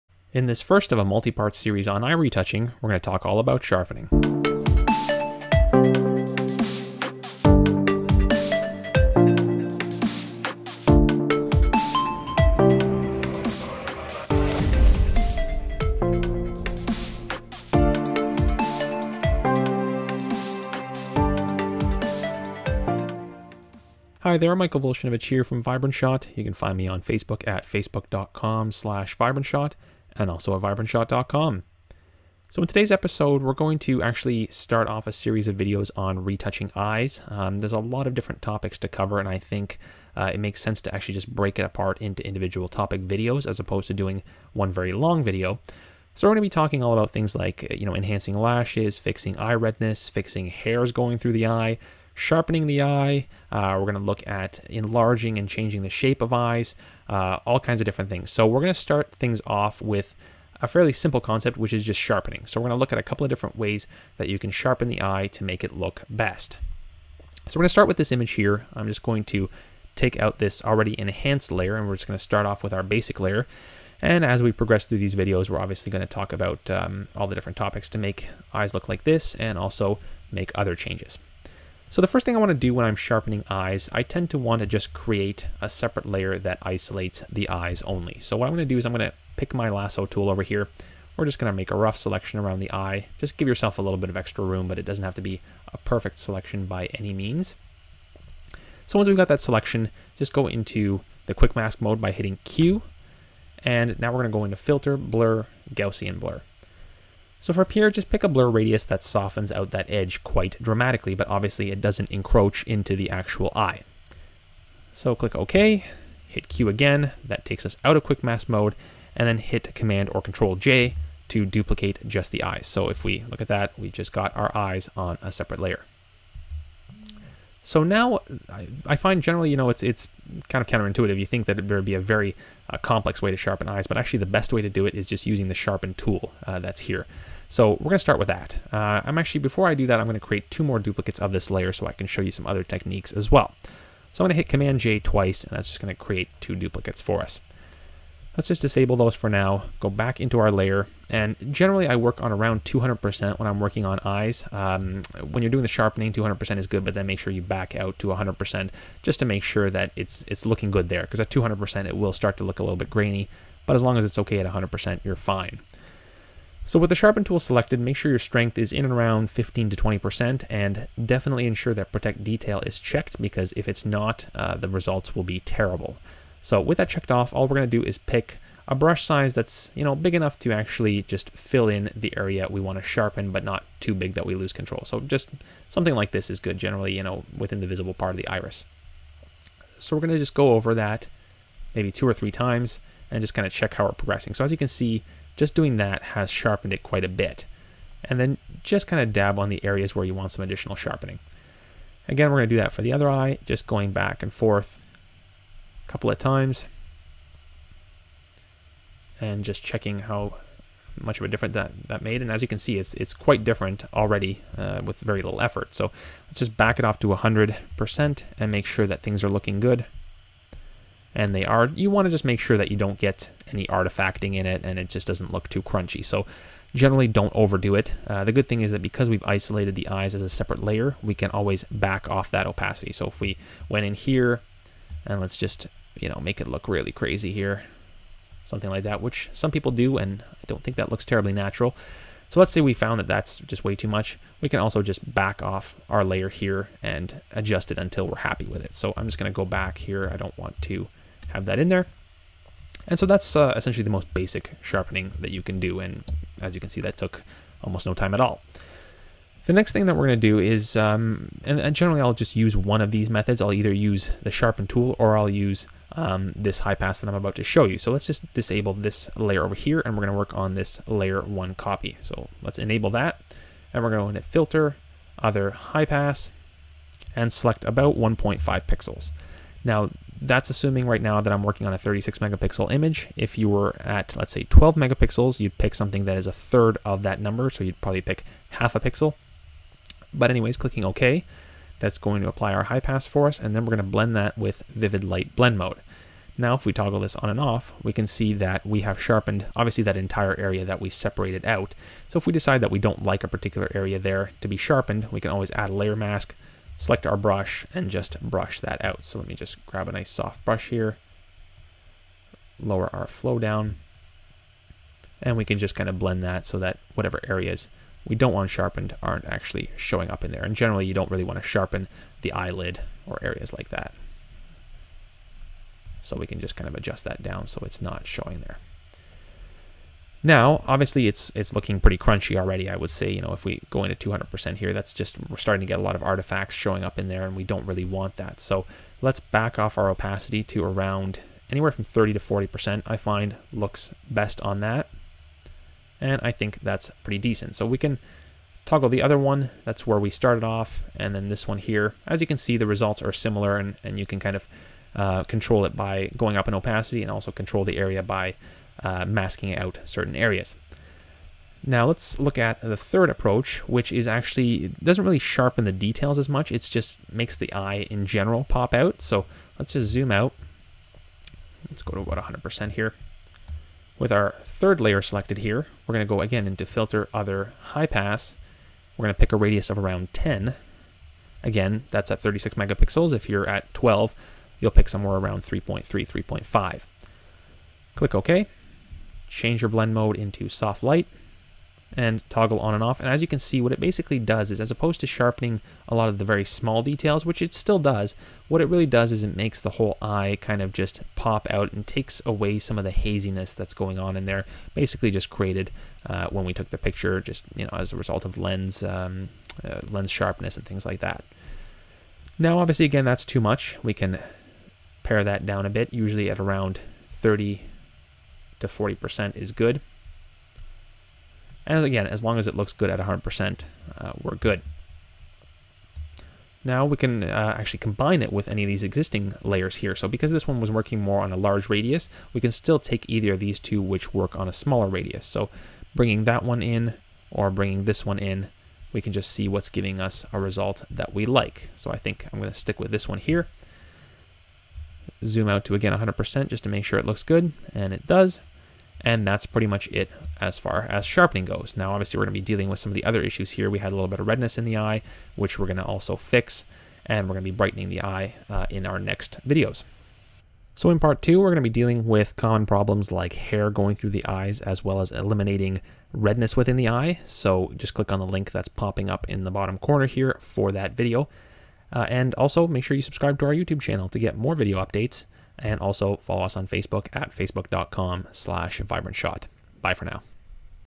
How to Sharpen Eyes in Photoshop - Retouching Eyes (Part 1)
In this first of a multi-part series on eye retouching we'll be looking at a few approaches to sharpening eyes.